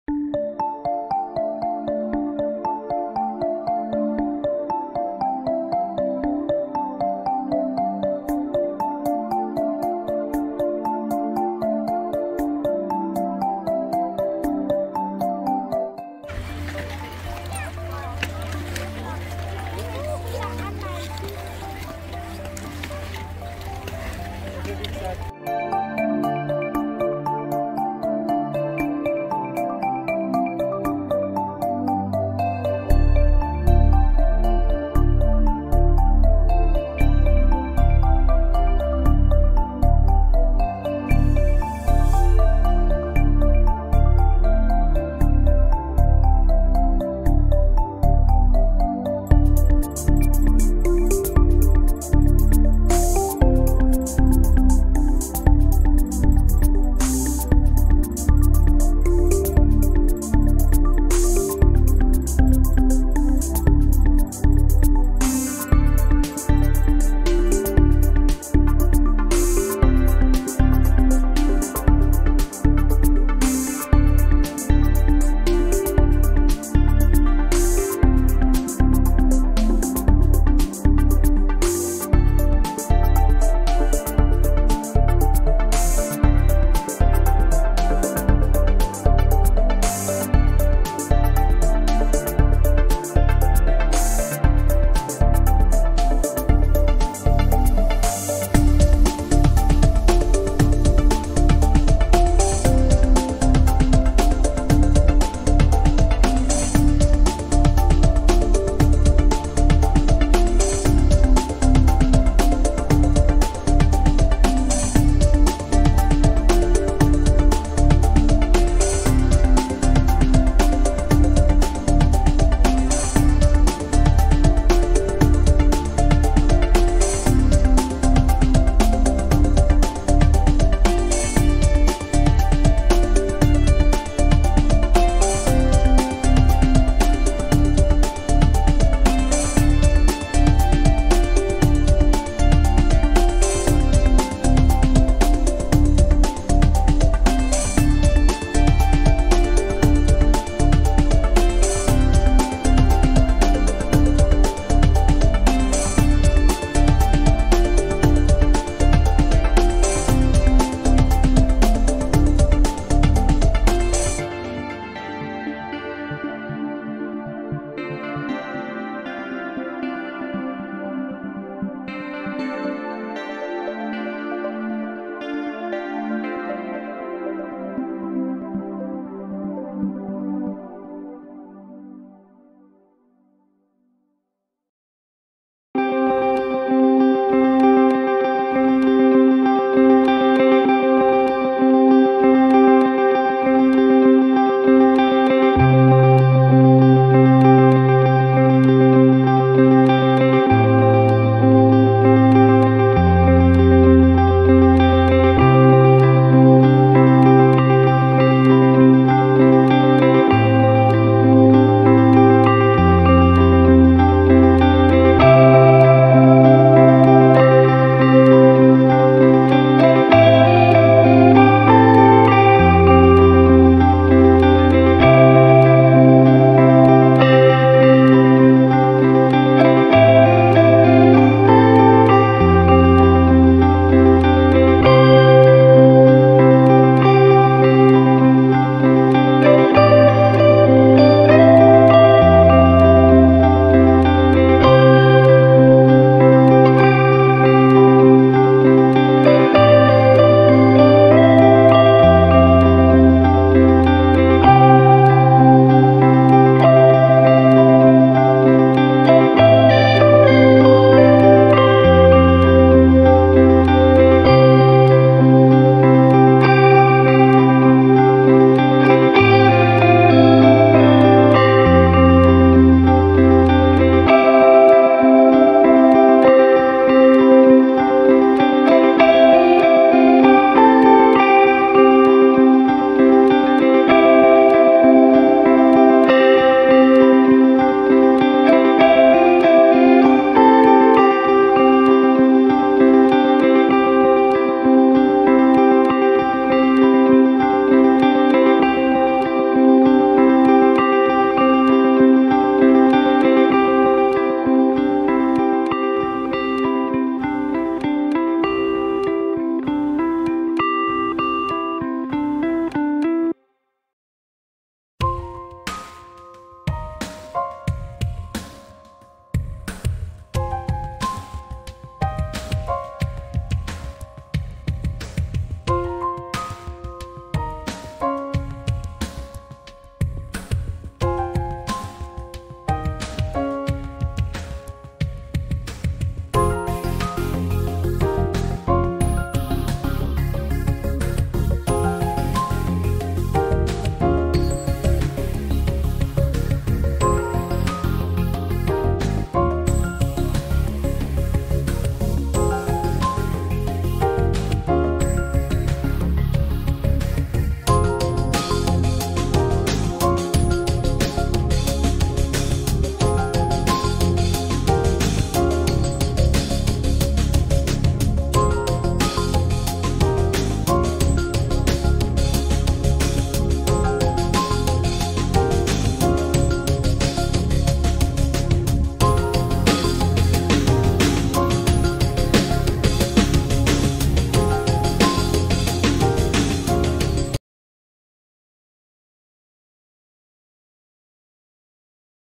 Auf dem Höhenwanderweg von der Söllereckbahnstation ins Kleinwalsertal
Fotos/Video mit Samsung Note 10+, unbearbeitet
Software: AquaSoft Diashow Easy (Mir zu easy, zu wenig Einflussmöglichkeiten)